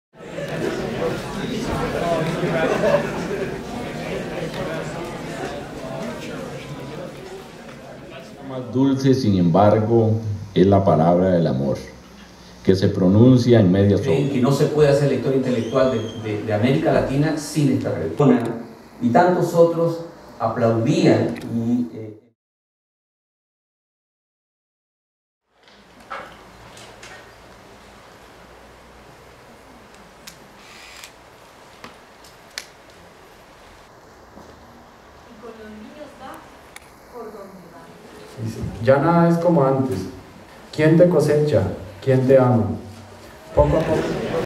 Día de la Poesía
Actividades realizadas en la Universidad Nacional relacionadas con el Día de la Poesía, 23 de  marzo.